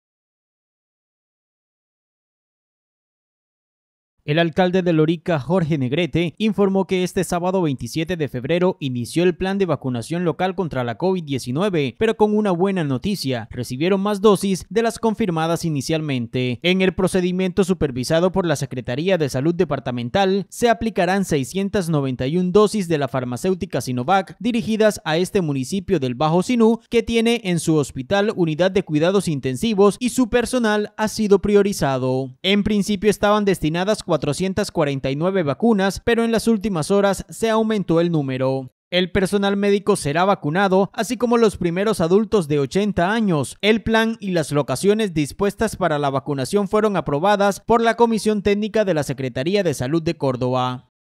A Lorica le aumentaron el despacho de dosis contra la Covid-19
EnVideo 🎥 El alcalde de Lorica, Jorge Negrete informó que este sábado 27 de febrero inició el plan de vacunación local contra la covid-19, pero con una buena noticia, recibieron más dosis de las confirmadas inicialmente.